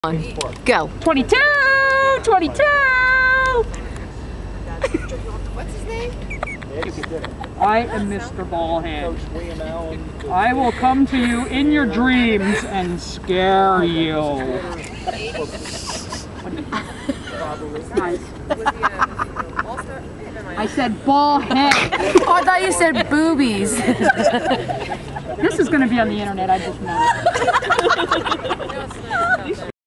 MR BALLHEAD
Introducing...Mr. Ballhead...self proclaimed mascot of the EWSL